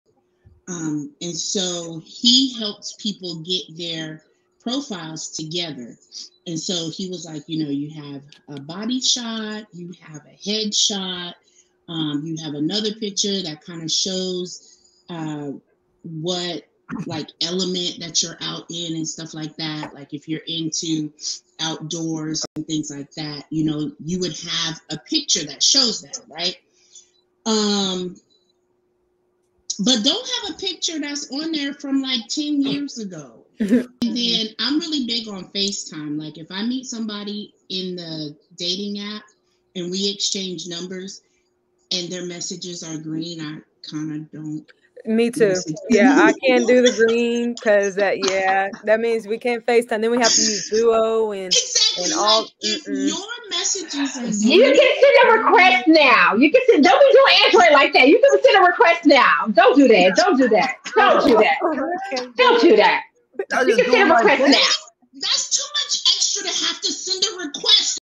The Ultimate Guide to Online Dating Profile Picture Tips & FaceTime Secrets
Unlock your online dating potential with our Ultimate Guide to Online Dating: Profile Picture Tips & FaceTime Secrets! Discover the secrets to choosing the perfect profile picture, learn how to shine on FaceTime dates, and make a lasting impression. Whether you're new to online dating or looking to up your game, our expert tips will boost your confidence and success.